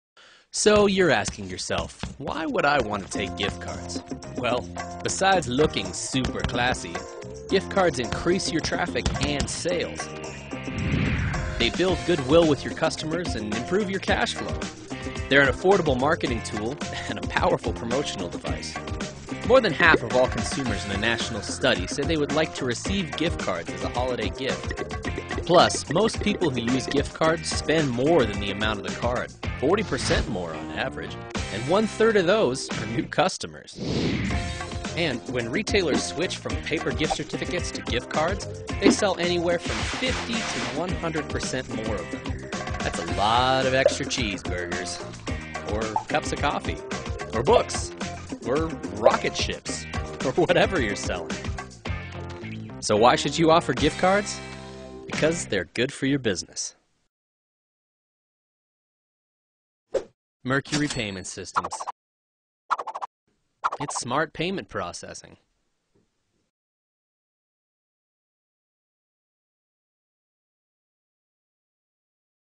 Why Are Gift Cards Useful
It seems like everyone uses gift cards nowadays. Why should your small business carry them? Mercury Payment Systems has the answer! Mercury Payment Systems Gift Cards are certified with Digital Dining Restaurant POS software.